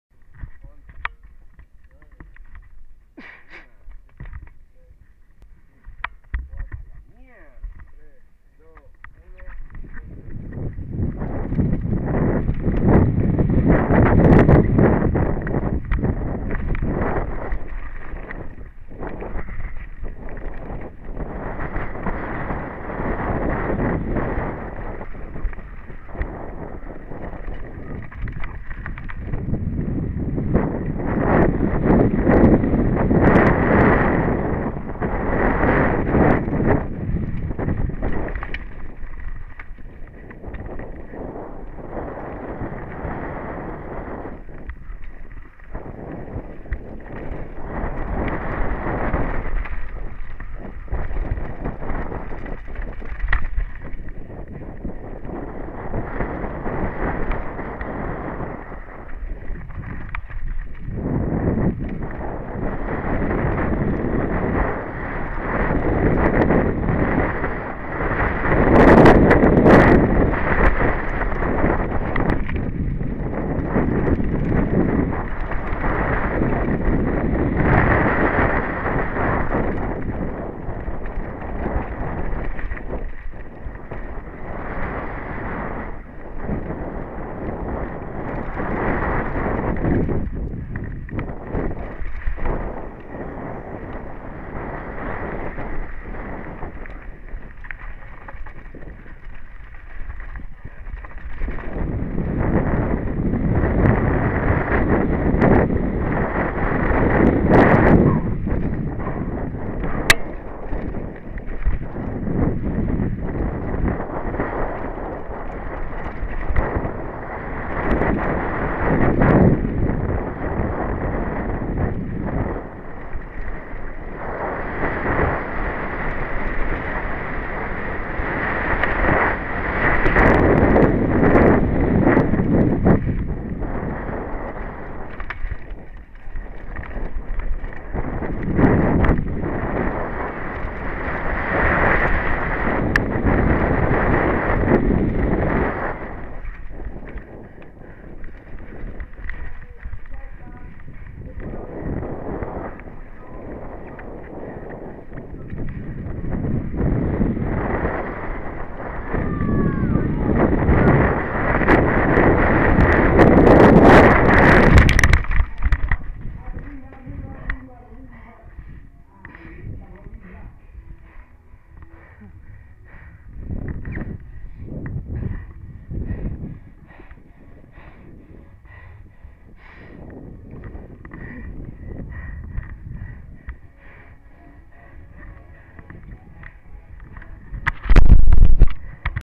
Clasificacion ultima fecha DHT 2014 - Gustavo Cisneros primera persona
En el Cadillal con una fuerte caida en la ultima curva